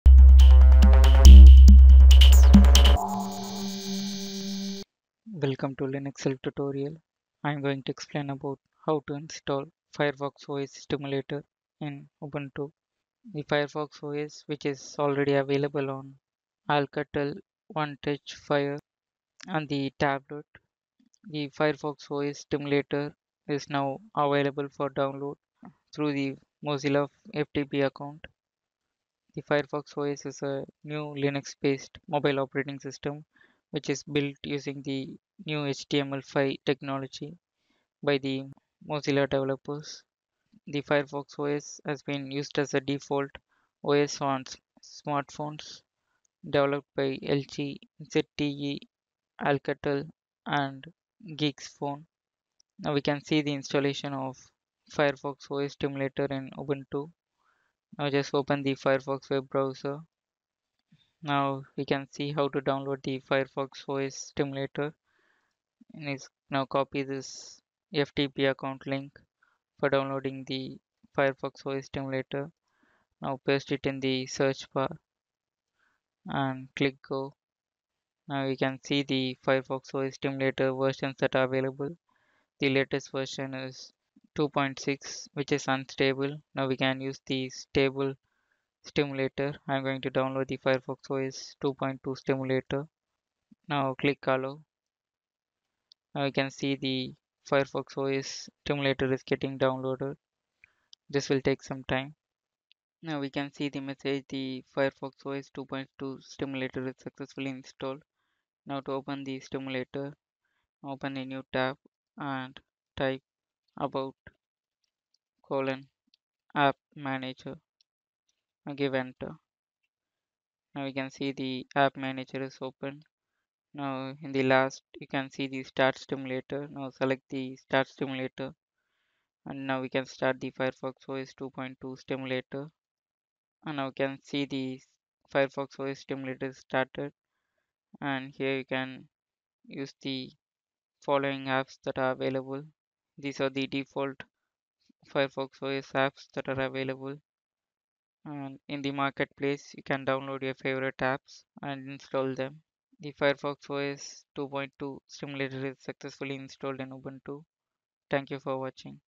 How to install Firefox OS Simulator in Ubuntu
This video explains the installation of Firefox OS simulator on Ubuntu. Firefox OS is a new Linux based mobile operating system, built by using the new HTML5 technology by the Mozilla developers.